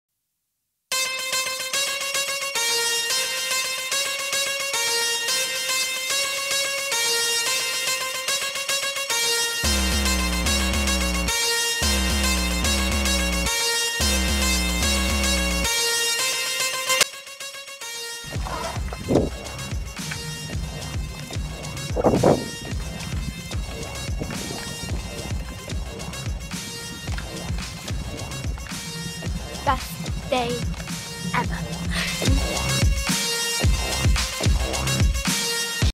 Maisie Williams is Rollerblading in California
Maisie Williams has the 'best day ever' rollerblading along Venice Beach in California